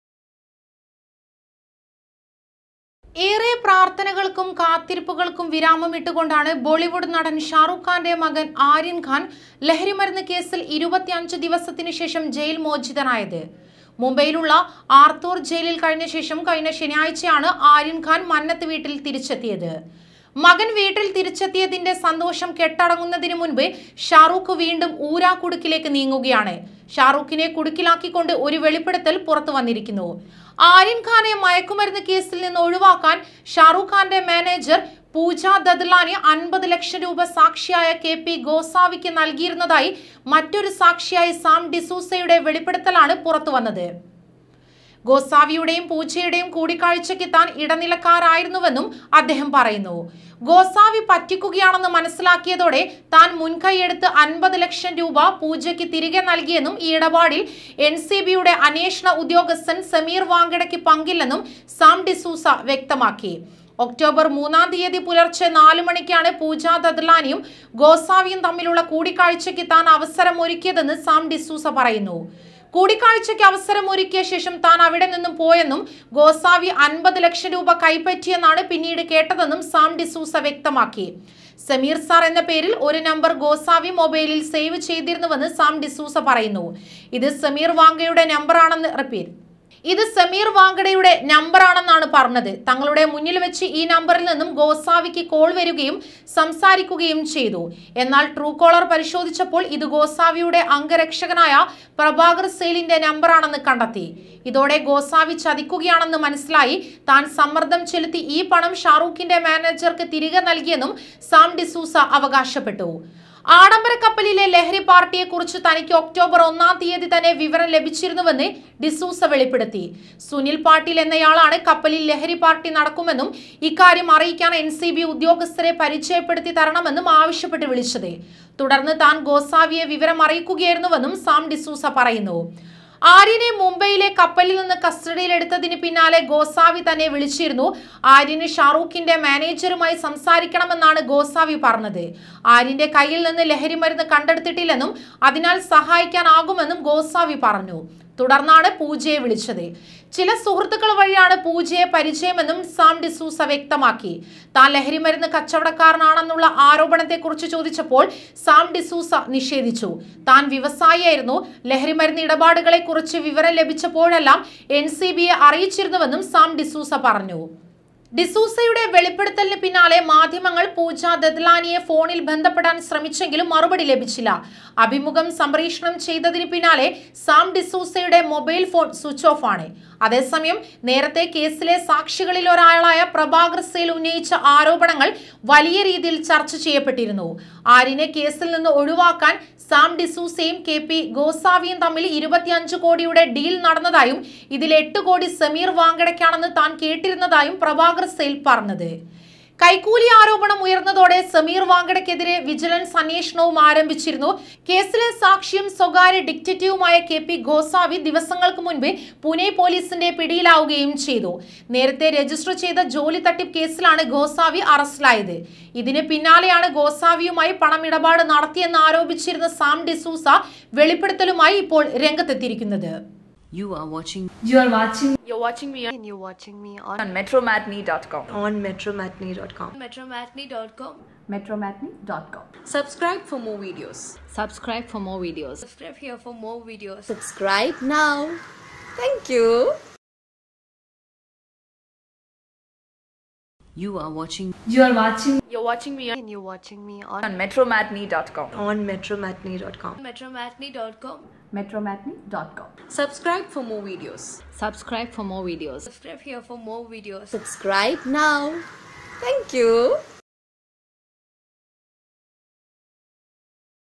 Aryan-khan I ഒടുവിൽ വമ്പൻ ട്വിസ്റ്റ്
ആര്യൻഖാനെ മയക്കുമരുന്നുകേസിൽ നിന്നൊഴിവാക്കാൻ ഷാരൂഖ്ഖാന്റെ മാനേജർ പൂജ ദദ്‌ലാനി 50 ലക്ഷം രൂപ സാക്ഷിയായ കെ.പി. ഗോസാവിക്ക് നൽകിയിരുന്നതായി മറ്റൊരു സാക്ഷിയായ സാം ഡിസൂസയുടെ വെളിപ്പെടുത്തലാണ് പുറത്തുവന്നത്